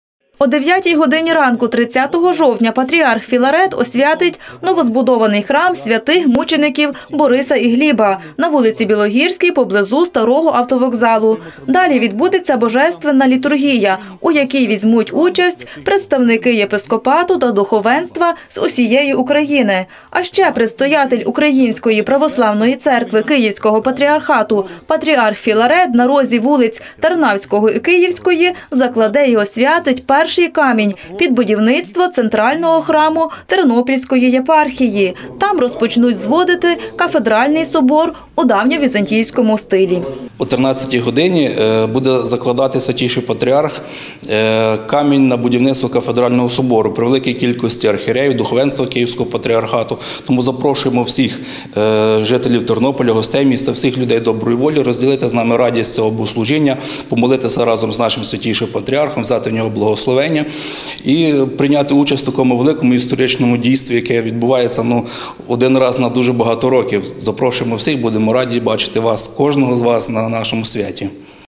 Святіший Патріарх Філарет відвідає Тернопільщину
30 жовтня 2016 року до Тернополя приїде Святіший Патріарх Філарет